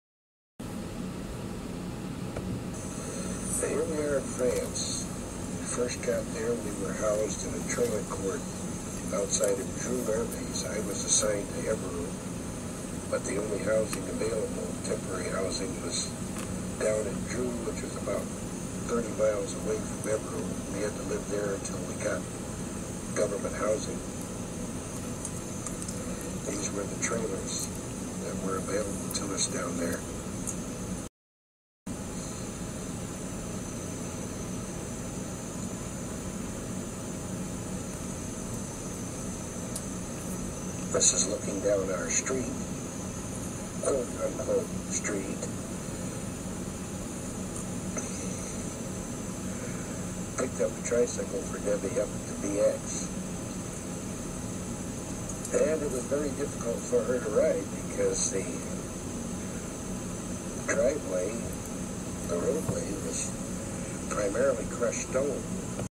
Dreux trailer park, France, 1961
If anyone lived on Dreux Air Base back in the late 50's or early 60's you might recognize this "housing area"